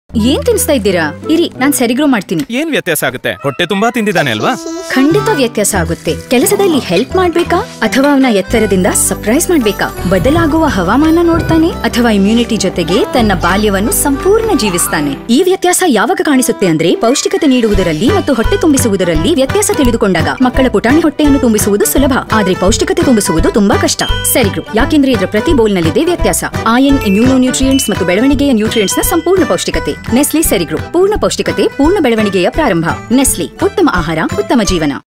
Ceregrow Farak Padta Hai | Kannada | 35 sec
Do we understand the difference between filling a child's tummy & filling it with nutrition? Watch the all new Ceregrow 'Farak Padta Hai' TVC to know the difference a bowl of Ceregrow can make leading to 'Poora Poshan' for the child & 'Poori Tasalli' for the mother.